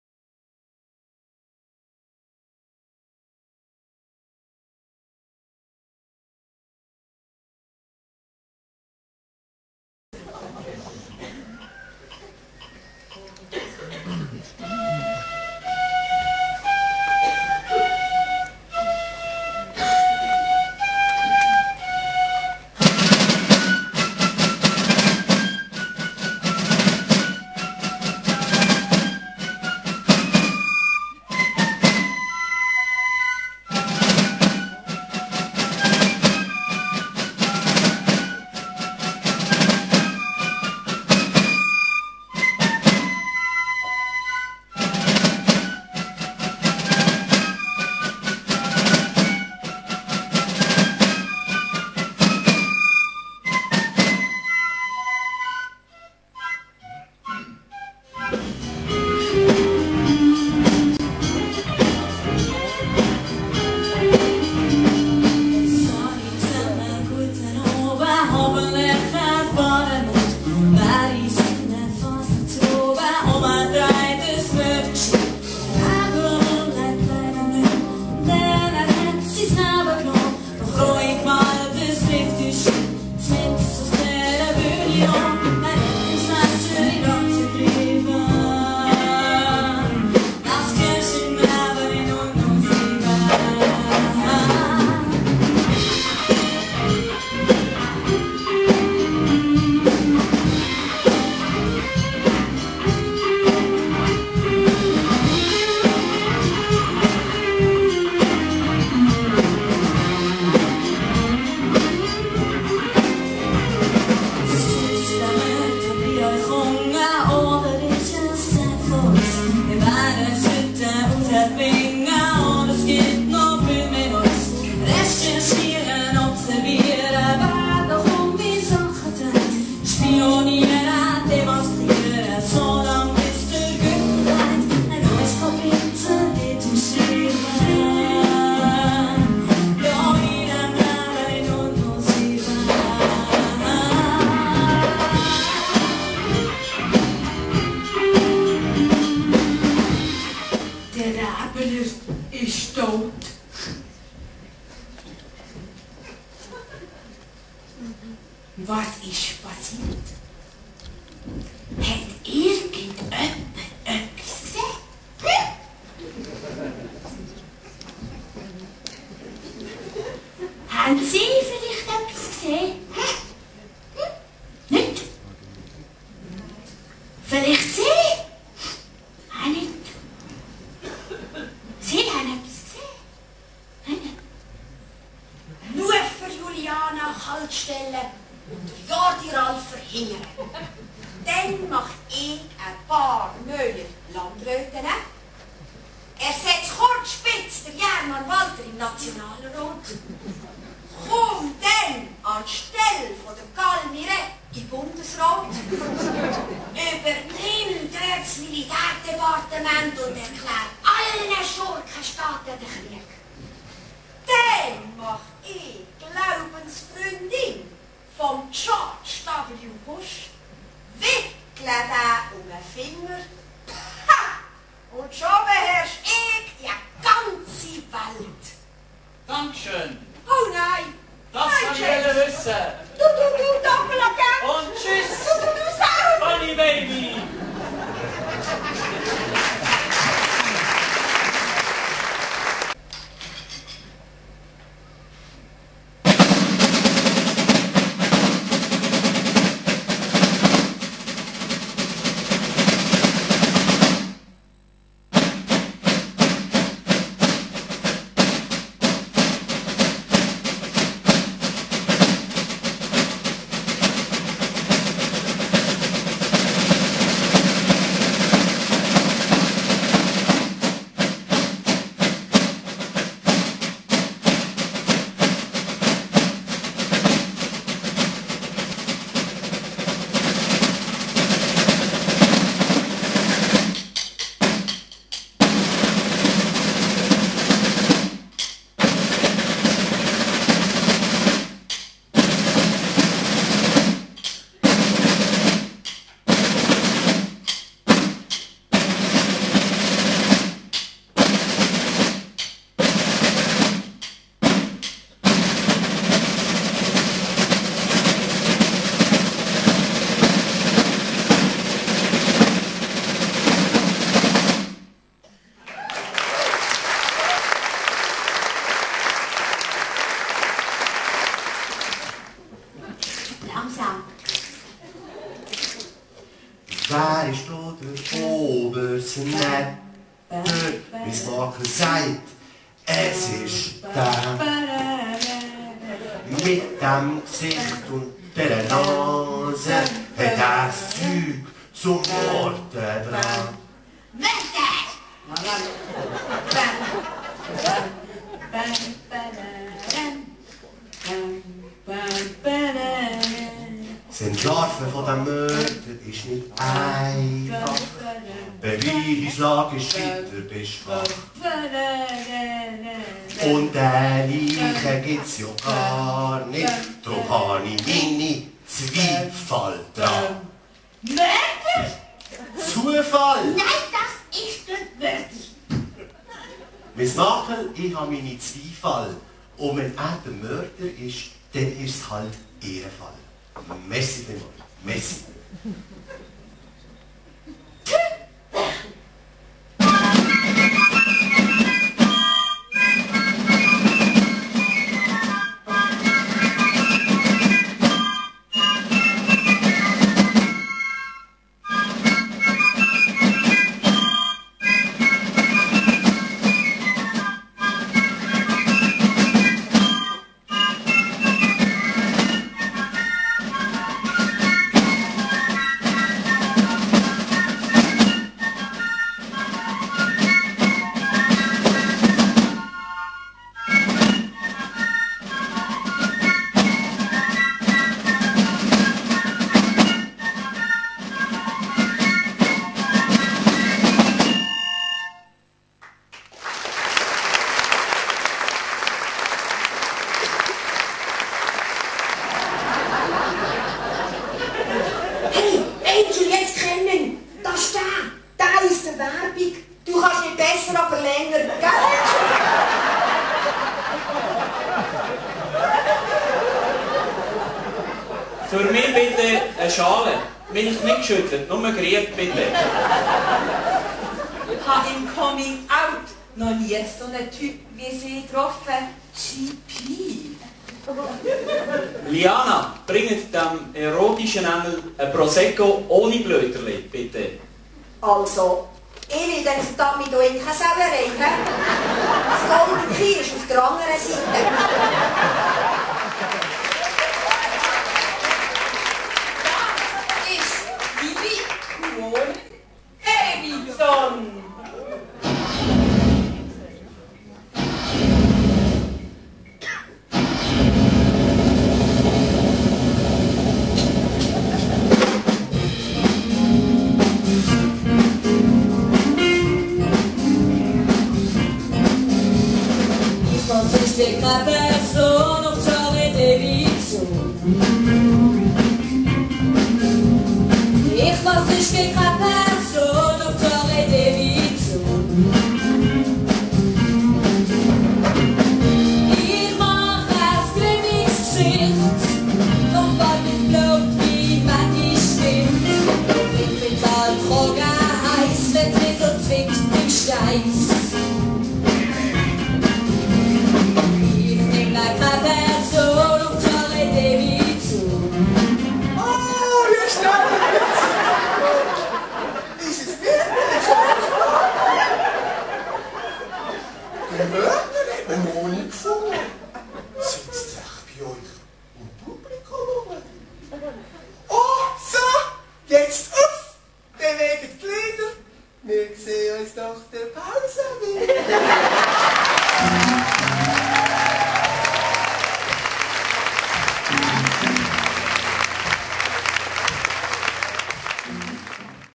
Räbeli 2007 / Querschnitt Teil 1
Das Fasnachts-Variété 2007, hier der erste Teil.